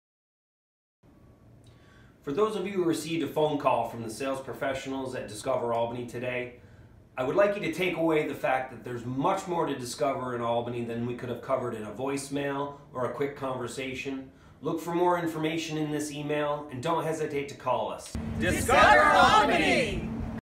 Winter Phone Blitz Follow up
Jay Cloutier, Director of Sales for Discover Albany, provides more information about our recent Winter Phone Blitz.